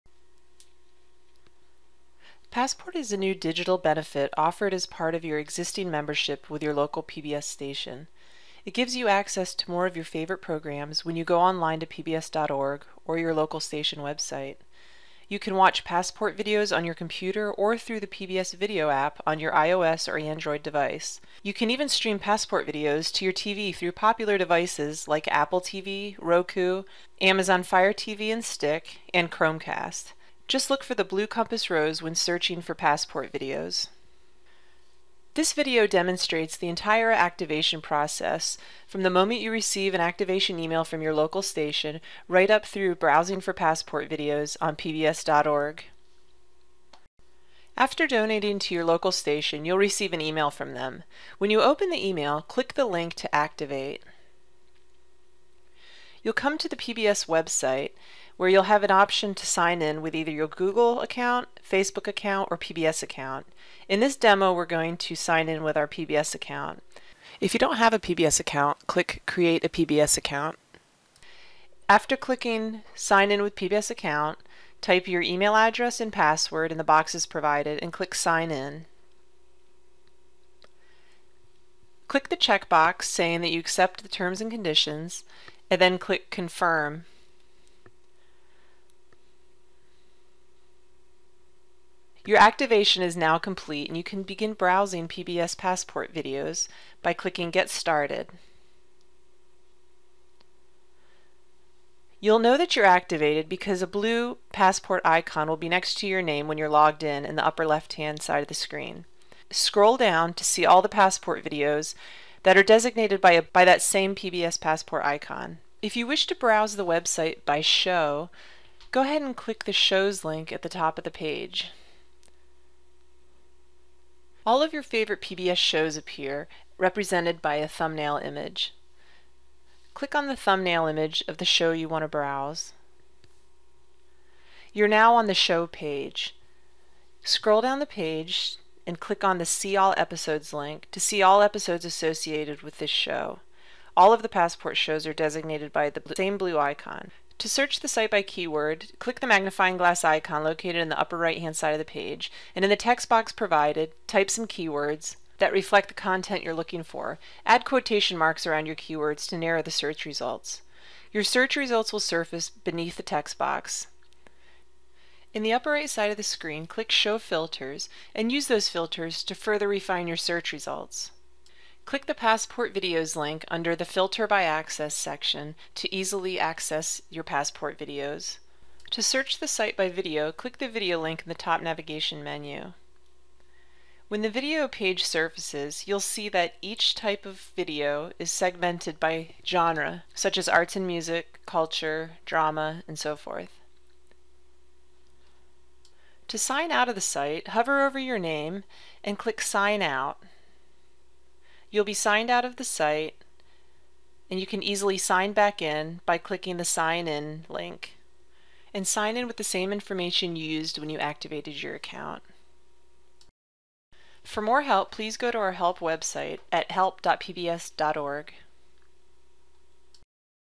How to Activate Passport
This video demonstrates how to activate your Passport member benefit from the moment you make a donation to your local station.